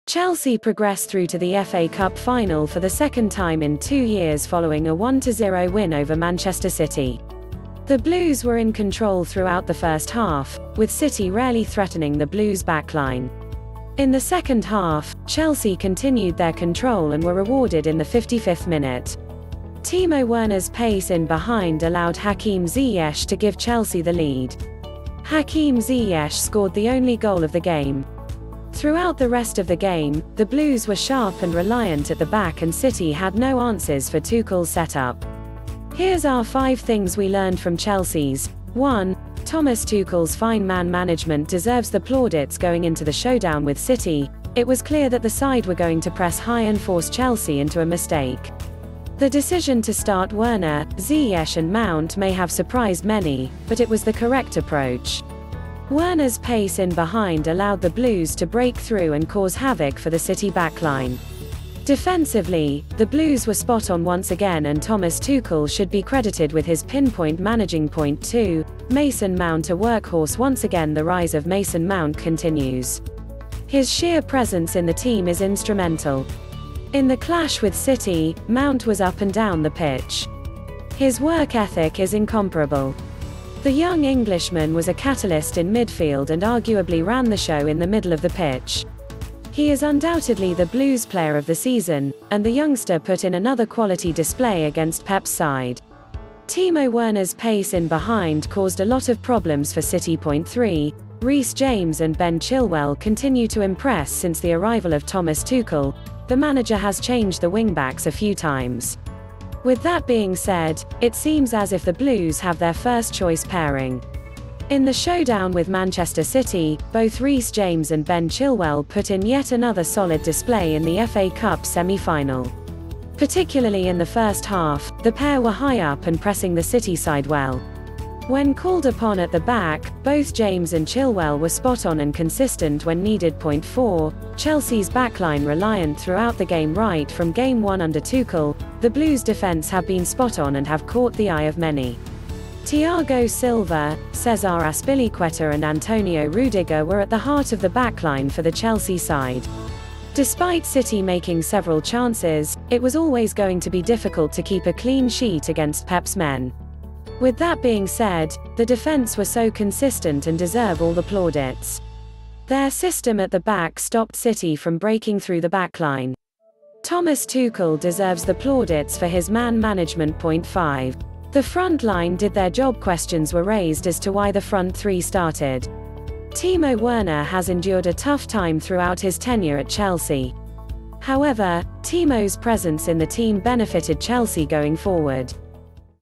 5 Things Learned Chelsea 1 0 Manchester City Emirates FA Cup
Chelsea progress through to the FA Cup Final for the second time in two years following a 1-0 win over Manchester City. The Blues were in control throughout the first half, with City rarely threatening the Blues' backline. In the second half, Chelsea continued their control and were rewarded in the fifty-fifth minute. Timo Werner's pace in behind allowed Hakim Ziyech to give Chelsea the lead. Hakim Ziyech scored the only goal of the game. Throughout the rest of the game, the Blues were sharp and reliant at the back and City had no answers for Tuchel's set-up. Here's our five things we learned from Chelsea's :1) Thomas Tuchel's fine man-management deserves the plauditsGoing into the showdown with City, it was clear that the side were going to press high and force Chelsea into a mistake. The decision to start Werner, Ziyech and Mount may have surprised many, but it was the correct approach. Werner's pace in behind allowed the Blues to break through and cause havoc for the City backline. Defensively, the Blues were spot on once again and Thomas Tuchel should be credited with his pin-point managing.2) Mason Mount a work-horse once againThe rise of Mason Mount continues. His sheer presence in the team is instrumental. In the clash with City, Mount was up and down the pitch. His work ethic is incomparable. The young Englishman was a catalyst in midfield and arguably ran the show in the middle of the pitch. He is undoubtedly the Blues' player of the season, and the youngster put in another quality display against Pep's side. Timo Werner's pace in behind caused a lot of problems for City.3) Reece James and Ben Chilwell continue to impressSince the arrival of Thomas Tuchel, the manager has changed the wingbacks a few times. With that being said, it seems as if the Blues have their first-choice pairing. In the showdown with Manchester City, both Reece James and Ben Chilwell put in yet another solid display in the FA Cup Semi-Final. Particularly in the first half, the pair were high up and pressing the City side well. When called upon at the back, both James and Chilwell were spot on and consistent when needed.4) Chelsea's backline reliant throughout the gameRight from game one under Tuchel, the Blues' defence have been spot on and have caught the eye of many. Thiago Silva, Cesar Azpilicueta and Antonio Rudiger were at the heart of the backline for the Chelsea side. Despite City making several chances, it was always going to be difficult to keep a clean sheet against Pep's men. With that being said, the defence were so consistent and deserve all the plaudits. Their system at the back stopped City from breaking through the backline. Thomas Tuchel deserves the plaudits for his man management.5) The frontline did their jobQuestions were raised as to why the front three started. Timo Werner has endured a tough time throughout his tenure at Chelsea. However, Timo's presence in the team benefitted Chelsea going forward.